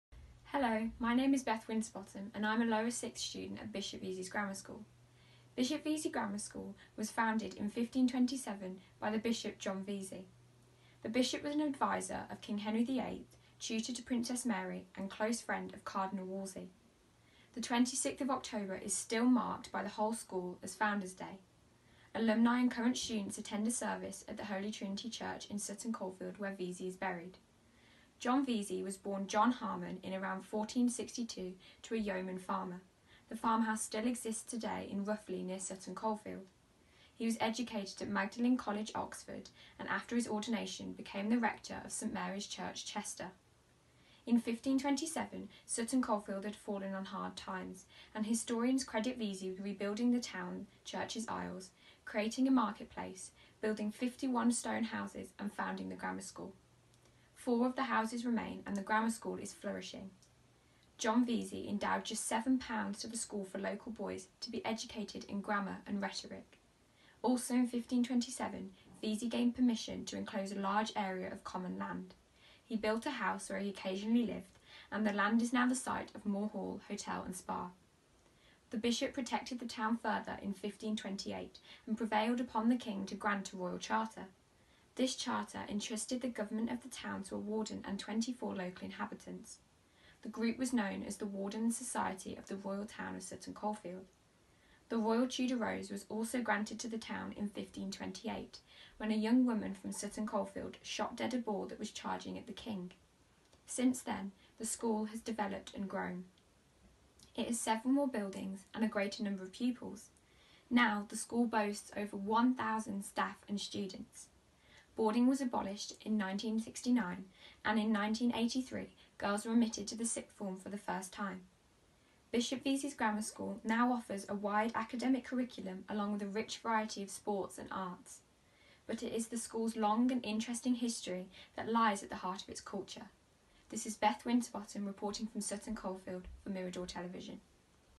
Beth in School
The long history of a famous English Grammar School is outlined by Beth Winterbottom who is a pupil there. She now reports for Mirador Television.